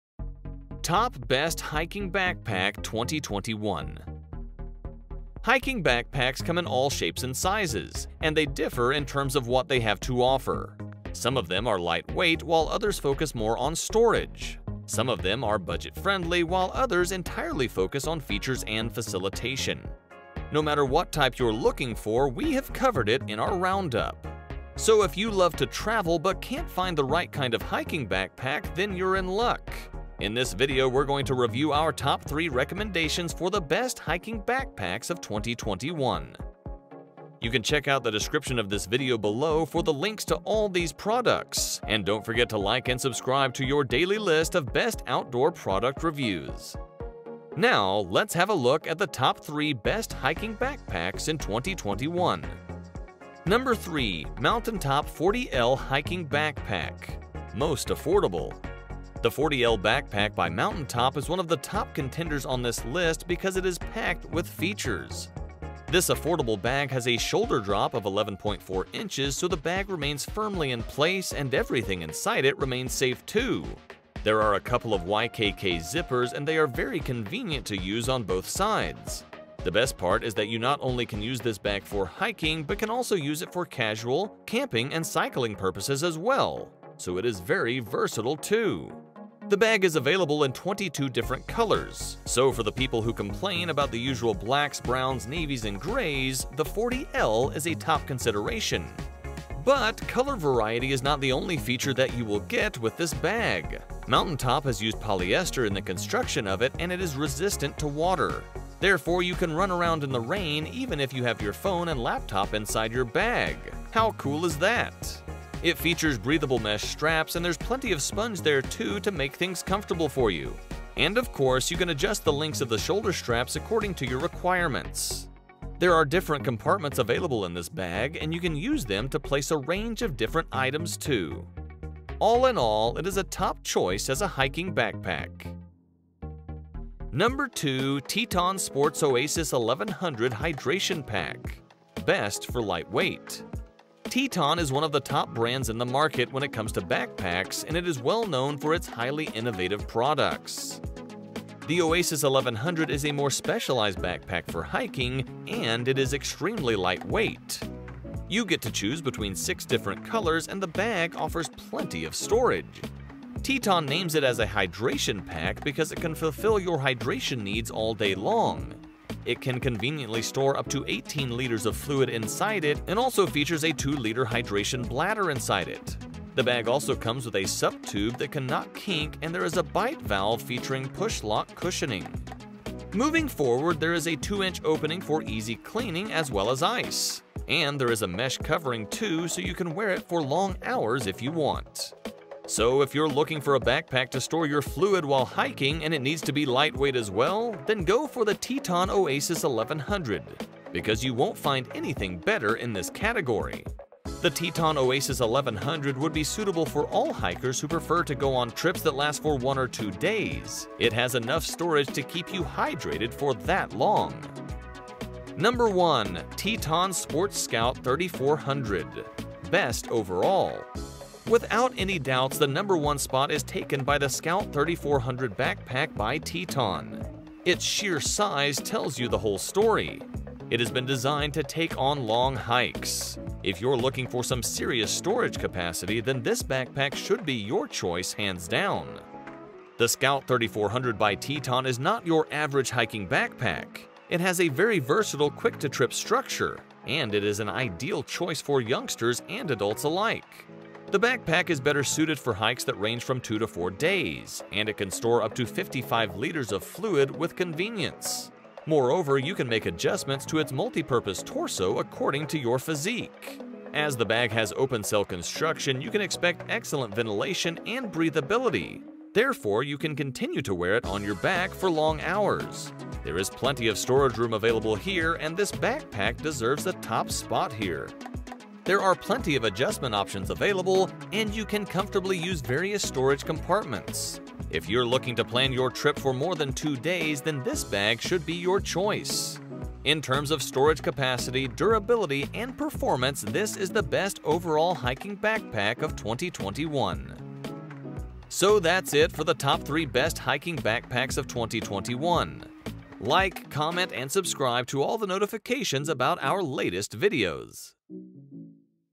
Top 3: Best Hiking Backpack of 2021!
Top 3 Of The Best Hiking Backpacks in 2021

✅ Links to The Best Hiking Backpacks 2021 We Featured In The Video:

✅ US Links
✔️ 3. MOUNTAINTOP 40L Hiking Backpack
✔️ 2. TETON Sports Oasis 1100 Hydration Pack
✔️ 1. TETON Sports Scout 3400

Top Best Hiking Backpack 2021

3. MOUNTAINTOP 40L Hiking Backpack (Most Affordable)
2. TETON Sports Oasis 1100 Hydration Pack (Best For Lightweight)
1. TETON Sports Scout 3400 (Best Overall)